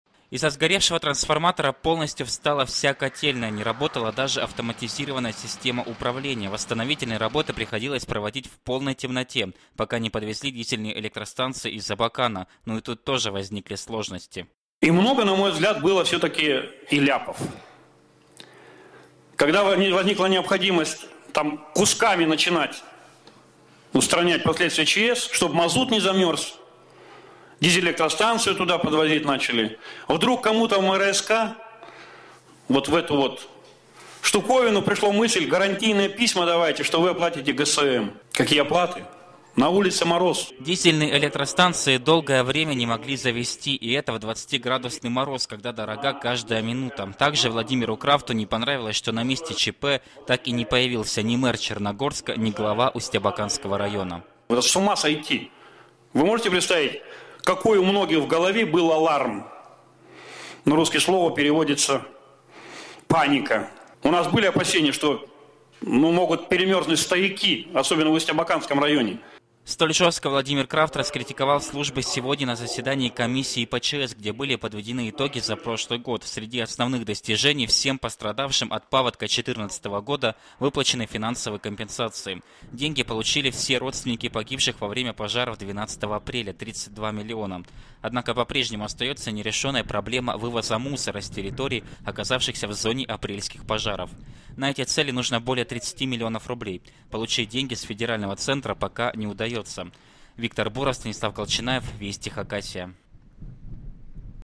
В правительстве прошло заседание комиссии по ЧС 22.01.2016
Резкой критике подвергли работу служб, которые устраняли последствия коммунального ЧП в Черногорске. Сегодня в правительстве прошло заседание комиссии по ЧС. На нем и обсудили итоги ликвидации  аварии, из-за которой  без тепла   могли остаться  сотни многоэтажек и десятки соцобъектов.